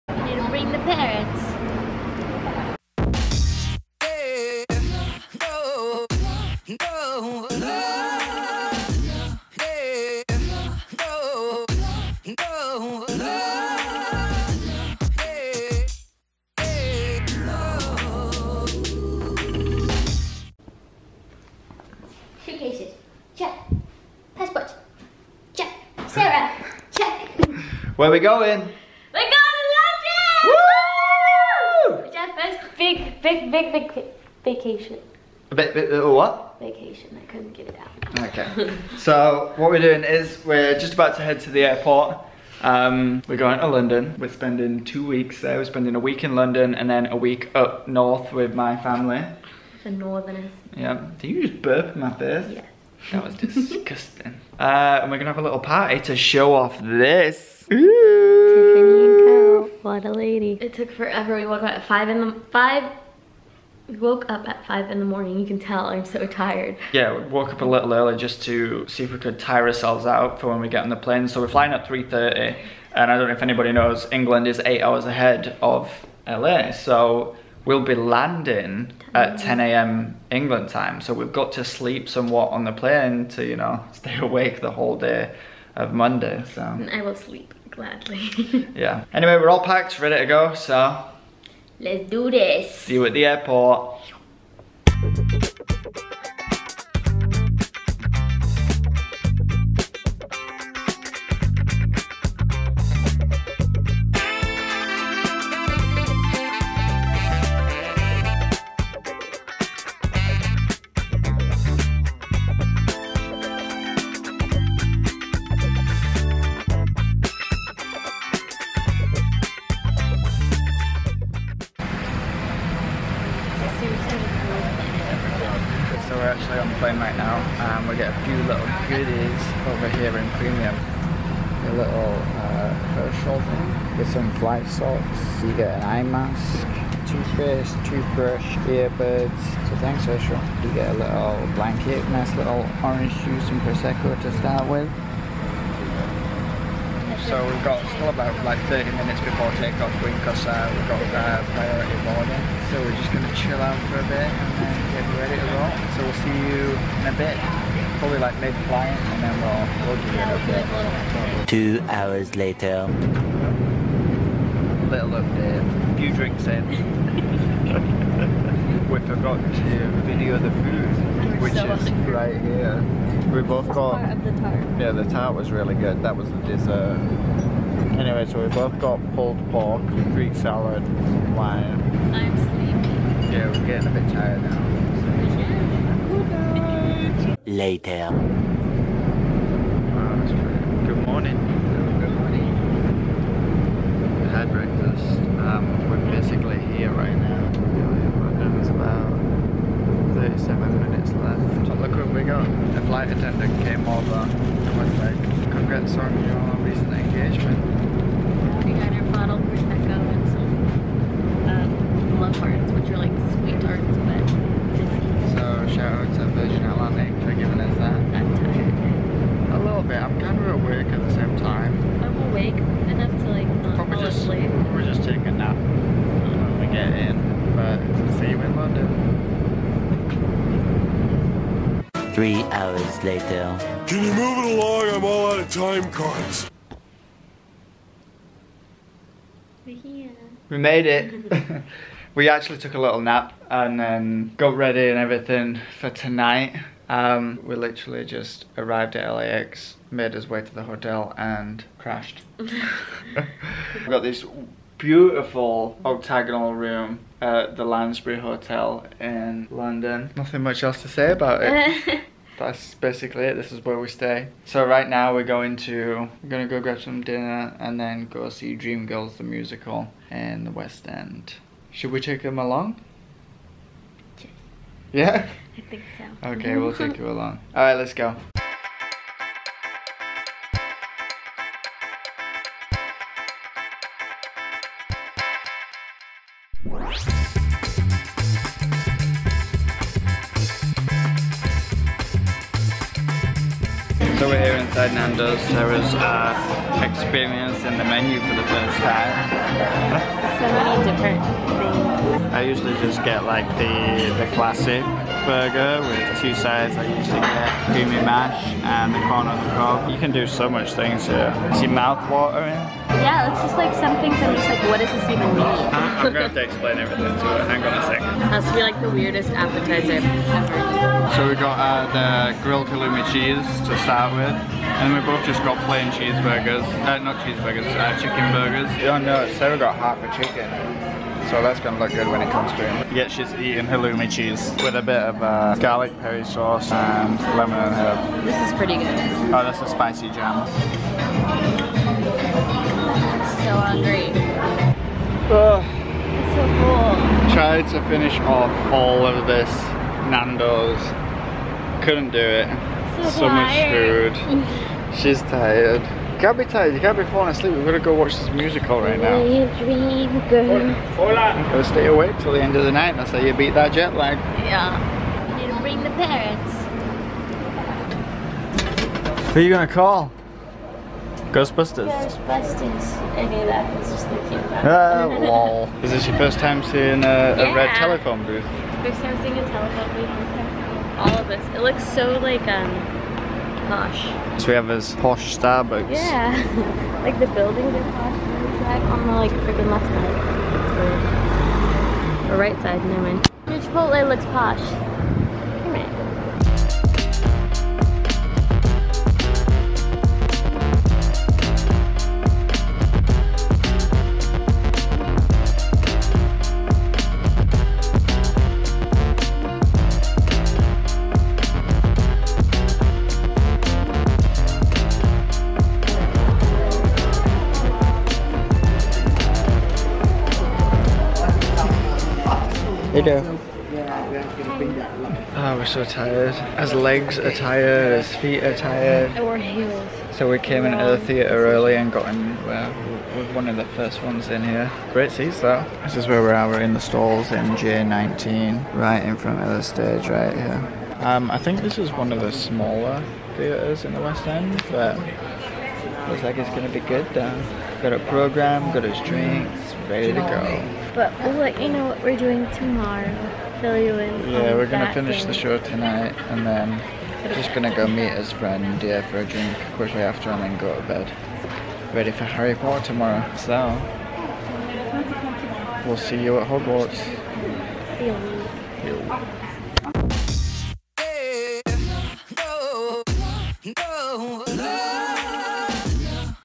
LONDON VLOG PART 1 | NANDOS & DREAMGIRLS
LONDON VLOG PART 1 | NANDOS & DREAMGIRLS - So we finally took our first International Vacation after announcing our engagement! In part 1, we fly from LAX to London Heathrow and begin our journey across the pond!

▶︎ Nathan: nafboy
▶︎ Sarah: sdanielleo

Nathan, Sarah & Harvey xx